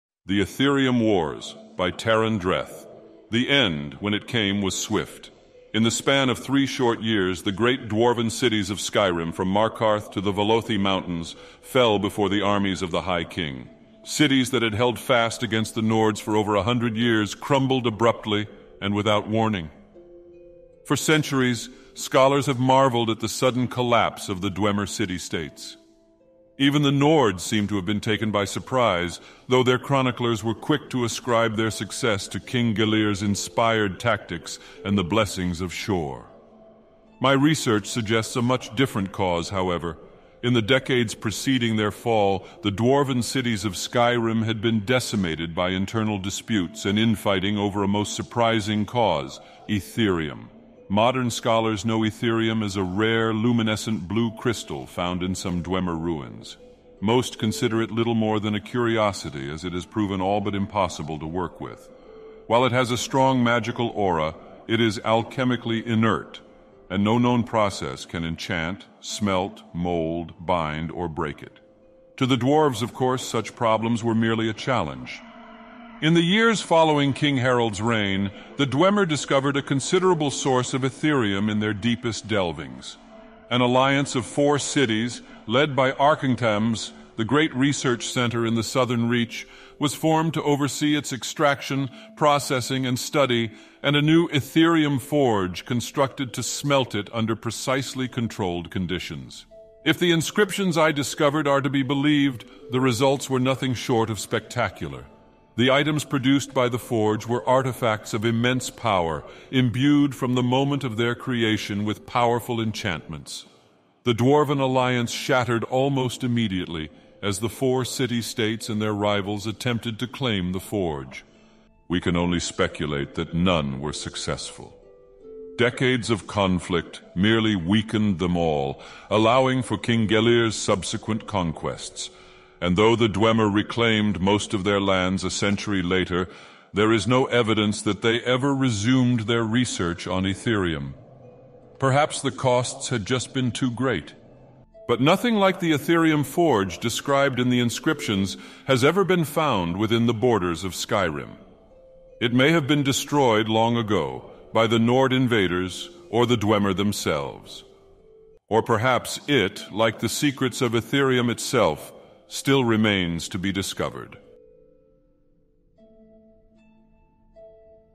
The Aetherium Wars: Read by Dagoth Ur
The end, when it came, was swift. In the span of three short years, the great dwarven cities of Skyrim, from Markarth to the Velothi Mountains, fell before the armies of the High King. Cities that had held fast against the Nords for over a hundred years crumbled abruptly and without warning.

For centuries, scholars have marveled at the sudden collapse of the Dwemer city-states. Even the Nords seem to have been taken by surprise, though their chroniclers were quick to ascribe their success to King Gellir's inspired tactics and the blessings of Shor.

My research suggests a much different cause, however. In the decades preceding their fall, the dwarven cities of Skyrim had been decimated by internal disputes and infighting over a most surprising cause: Aetherium.

Modern scholars know Aetherium as a rare, luminescent blue crystal found in some Dwemer ruins. Most consider it little more than a curiosity, as it has proven all but impossible to work with: while it has a strong magical aura, it is alchemically inert, and no known process can enchant, smelt, mold, bind, or break it.

To the dwarves, of course, such problems were merely a challenge. In the years following King Harald's reign, the Dwemer discovered a considerable source of Aetherium in their deepest delvings. An alliance of four cities, led by Arkngthamz, the great research center in the southern Reach, was formed to oversee its extraction, processing, and study, and a new 'Aetherium Forge' constructed to smelt it under precisely controlled conditions.

If the inscriptions I discovered are to be believed, the results were nothing short of spectacular: the items produced by the Forge were artifacts of immense power, imbued from the moment of their creation with powerful enchantments. The dwarven alliance shattered almost immediately, as the four city-states and their rivals attempted to claim the Forge.

We can only speculate that none were successful. Decades of conflict merely weakened them all, allowing for King Gellir's subsequent conquests. And though the Dwemer reclaimed most of their lands a century later, there is no evidence that they ever resumed their research on Aetherium. Perhaps the costs had just been too great.

But nothing like the Aetherium Forge described in the inscriptions has ever been found within the borders of Skyrim. It may have been destroyed long ago, by the Nord invaders or the Dwemer themselves. Or perhaps it, like the secrets of Aetherium itself, still remains to be discovered.